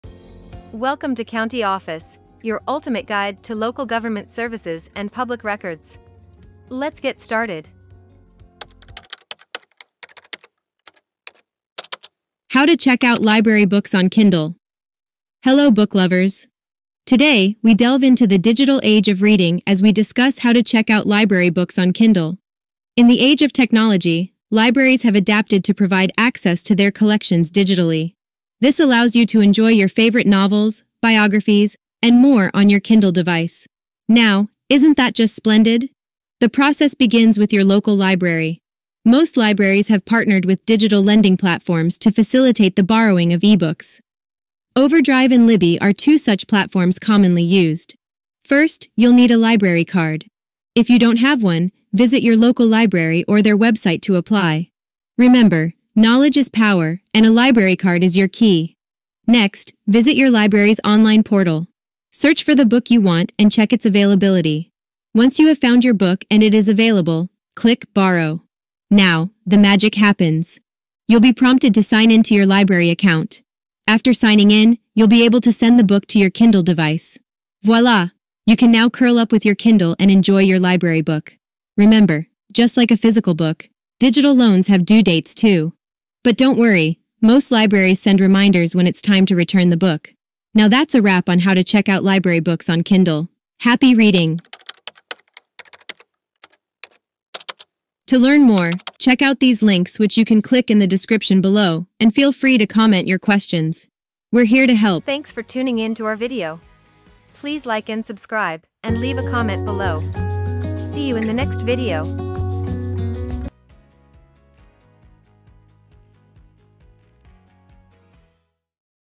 How To Check Out Library Books On Kindle? - CountyOffice.org
How To Check Out Library Books On Kindle? Ever wondered how to borrow library books on your Kindle? In this enlightening video, we'll walk you through the process of checking out library books on your Kindle device.

We'll start by discussing how libraries have adapted to the digital age. Most libraries have partnered with digital lending platforms like OverDrive and Libby, allowing you to borrow ebooks and read them on your Kindle.

We'll guide you through the steps of obtaining a library card if you don't already have one. Then we'll show you how to navigate your library's online portal, search for your desired book, and check its availability.

Once you've found your book, we'll explain how to 'borrow' it digitally. We'll also cover how to sign in to your library account and send the book to your Kindle device.

Remember, just like physical books, digital loans have due dates too! We'll touch on this and provide tips for managing your digital loans.

Whether you're a seasoned reader or new to the world of ebooks, this video is a must-watch.